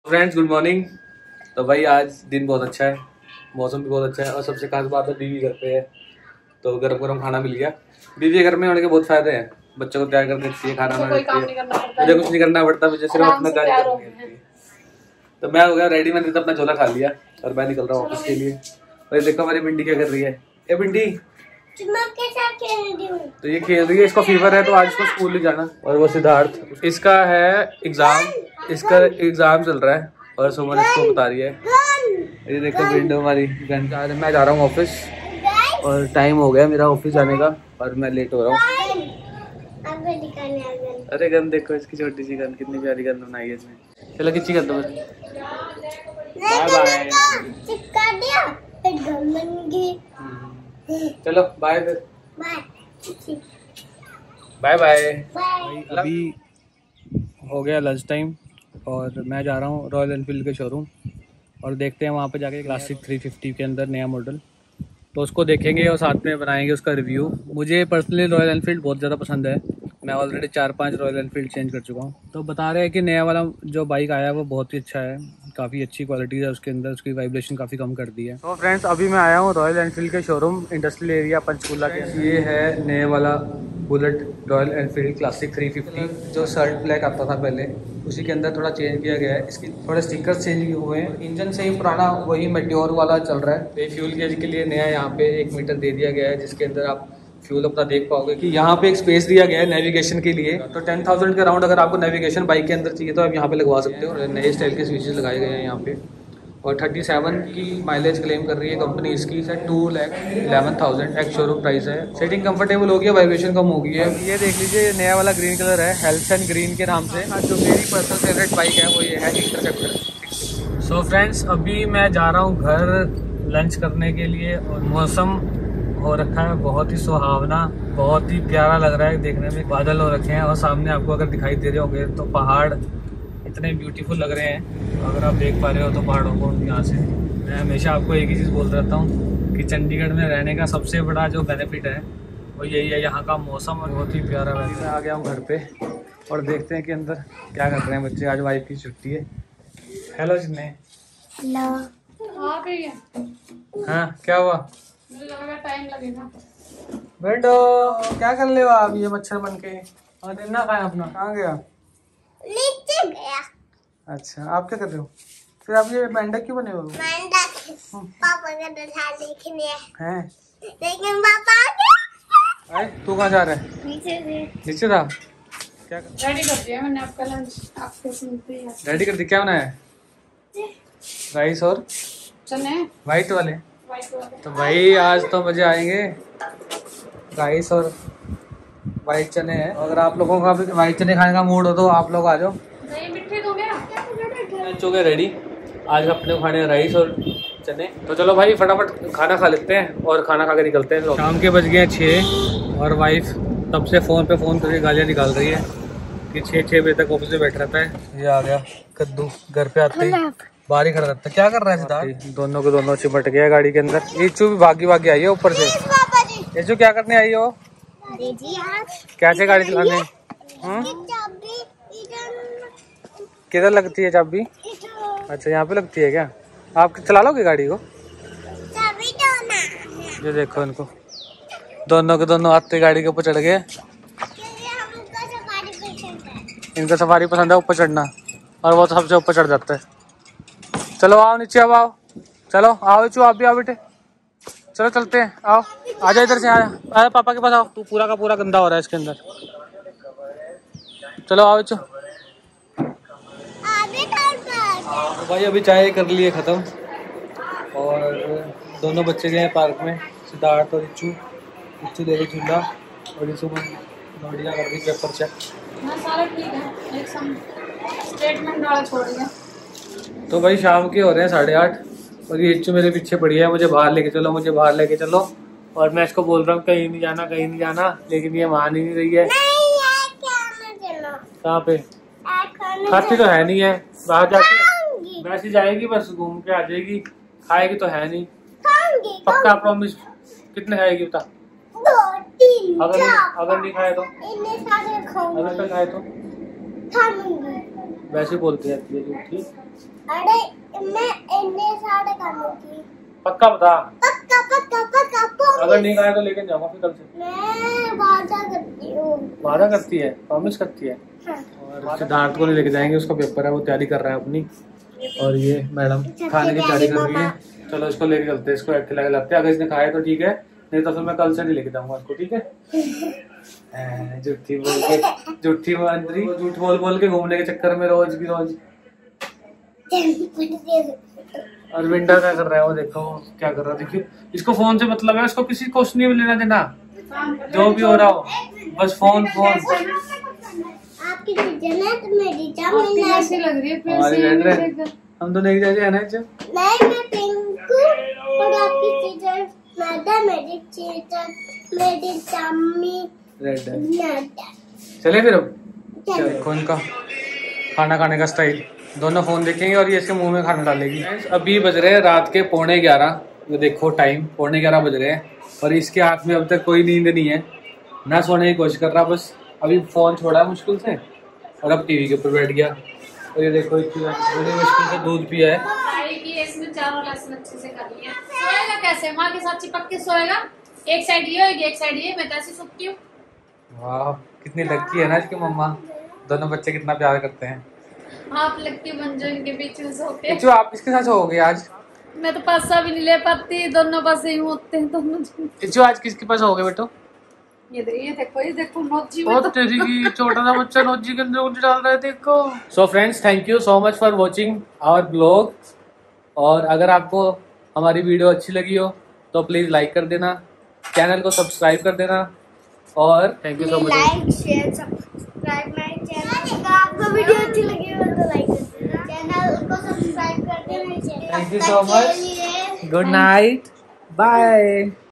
New Bullet-350 mast hai, Ye Dono Safari ke upar hi rehte hai.
New Bullet-350 mast hai. Ye dono safari ke upar hi chad jate hai.